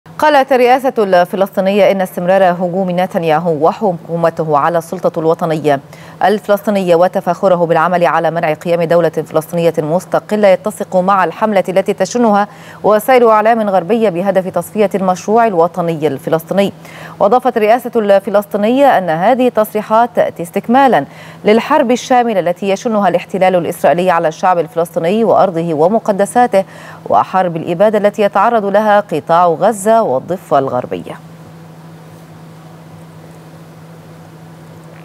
الرئاسة الفلسطينية: تصريحات نتنياهو حول منع قيام دولة فلسطينية تتسق مع الحملة الإعلامية الغربية
غرفة الأخبار| الرئاسة الفلسطينية: تصريحات نتنياهو حول منع قيام دولة فلسطينية تتسق مع الحملة الإعلامية الغربية بهدف تصفية المشروع الوطني الفلسطيني